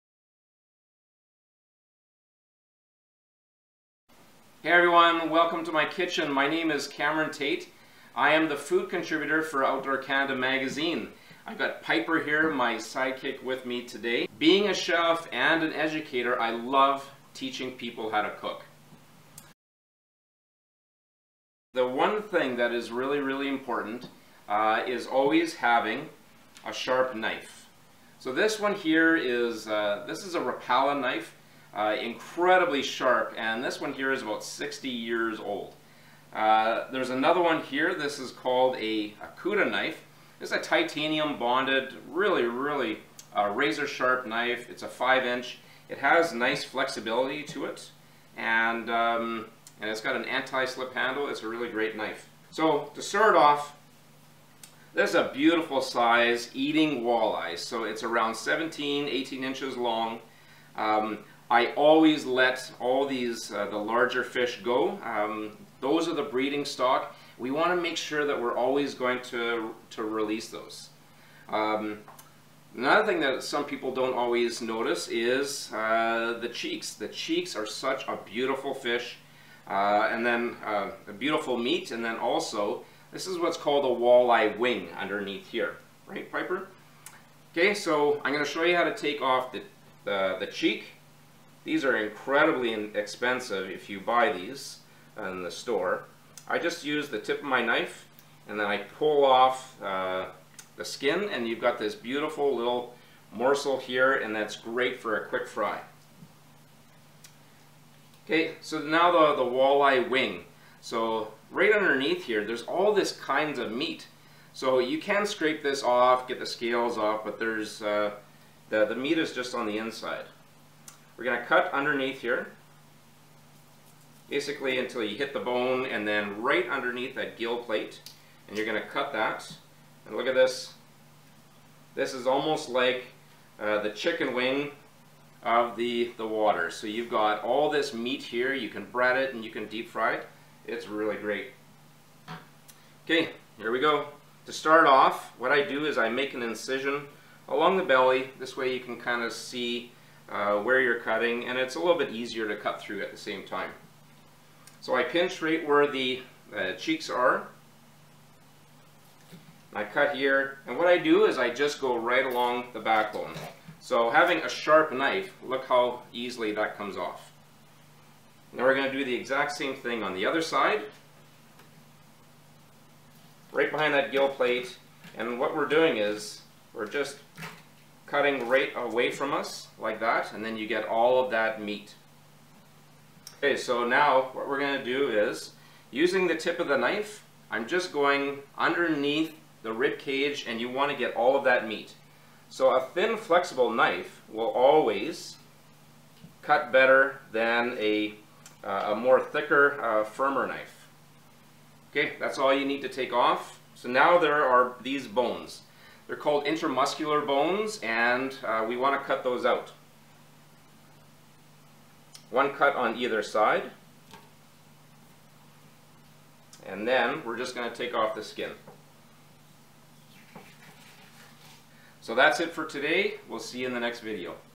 Wild Cuisine: How to get flawless walleye fillets
Outdoor Canada magazine food contributor and chef Cameron Tait shows how to get totally boneless walleye fillets. As well, he points out cuts of the fish that many people overlook, including the fish's cheeks and wings.